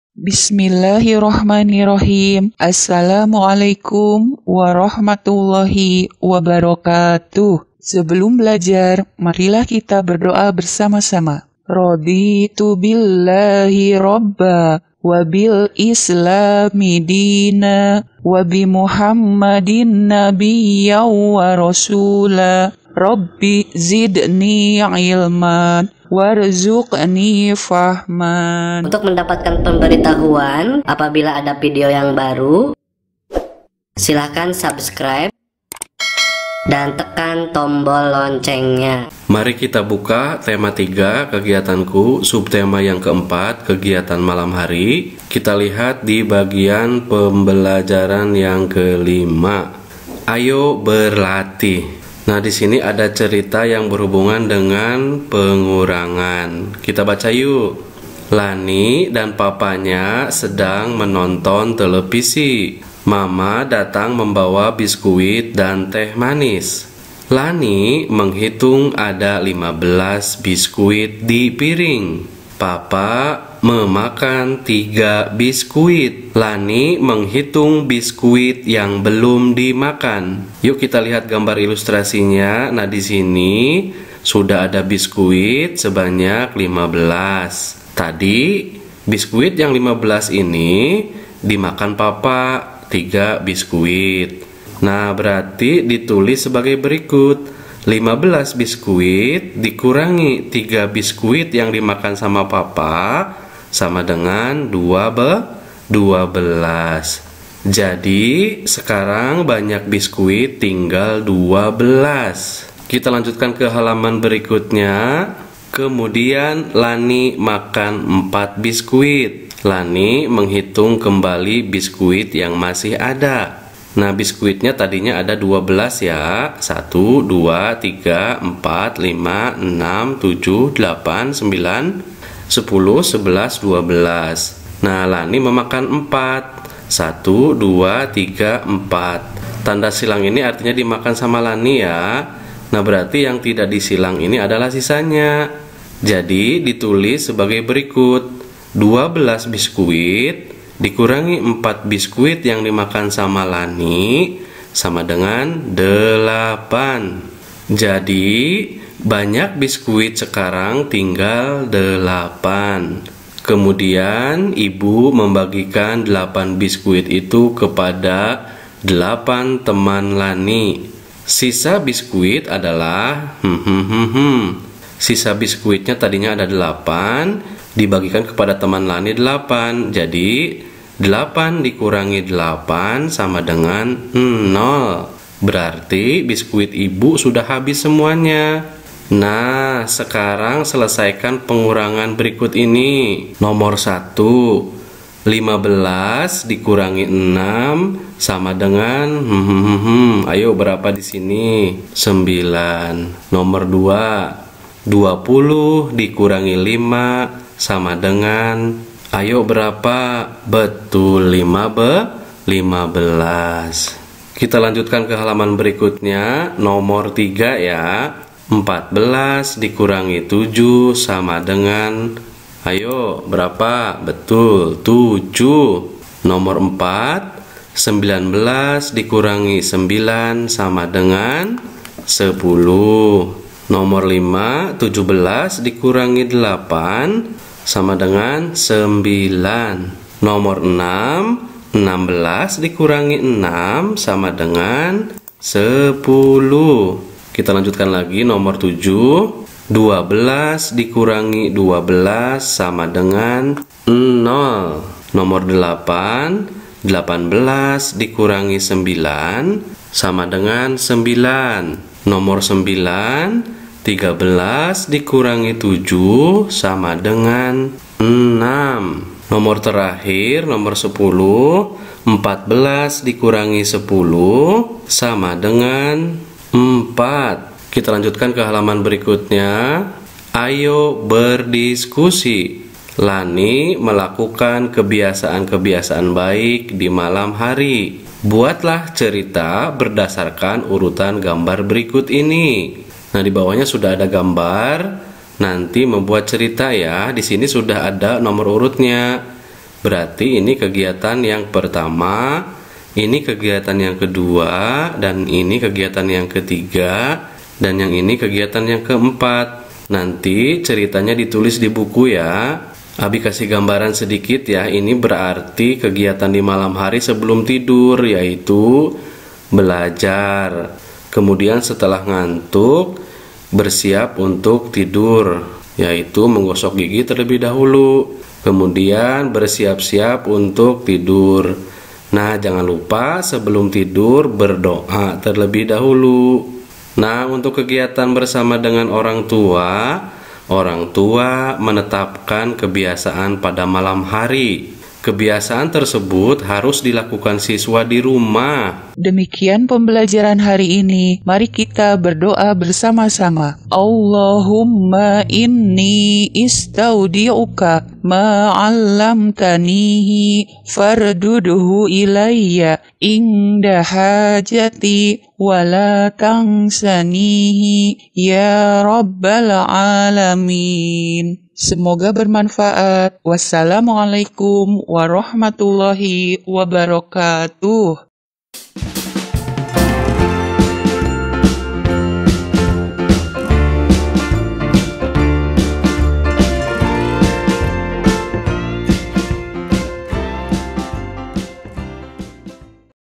PEMBELAJARAN KELAS 1 TEMA 3 SUBTEMA 4 PELAJARAN 5 DISERTAI PEMBAHASAN SOAL
Sebelum belajar Tema 3 Kegiatanku Subtema 4 Kegiatan Malam Hari Pembelajaran 5, jangan lupa sebelum dan sesudah belajar harus berdo'a supaya diberi kemudahan untuk mendapatkan ilmu. Selamat belajar! Jadilah penerus yang beriman dan bertaqwa menuju generasi emas 2045!

Sumber:
Buku Tematik Terpadu Kurikulum 2013 Revisi 2017
Kemendikbud RI